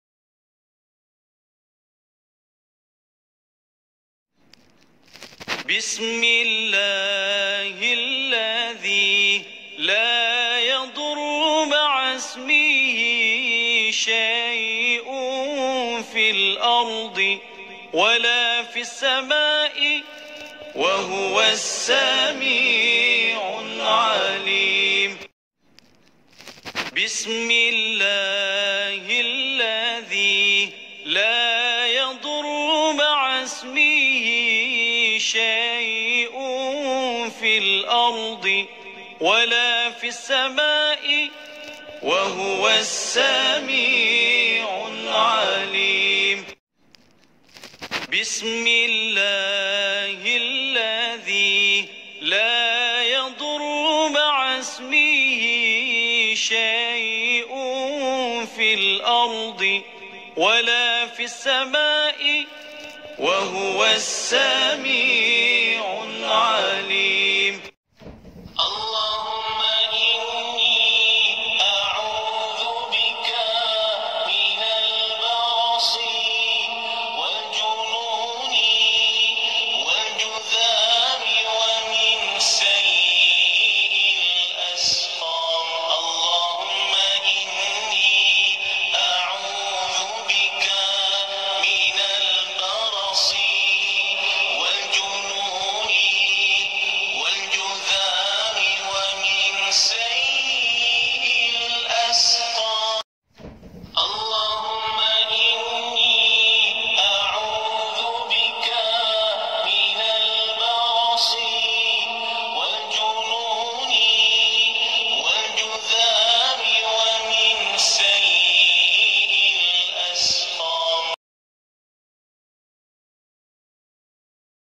DOA PERLINDUNGAN DARI WABAK
Mari beramal dengan doa ini, pagi dan petang. Moga kita semua dijauhi daripada wabak penyakit. Inshaa Allah.